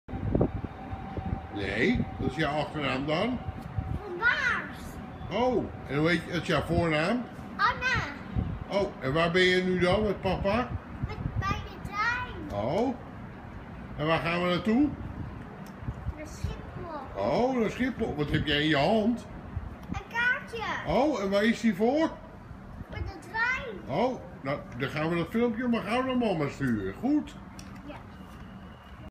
Anna van Baars neemt de trein
Anna samen met pappa nemen de trein naar Schiphol
7 januari 2015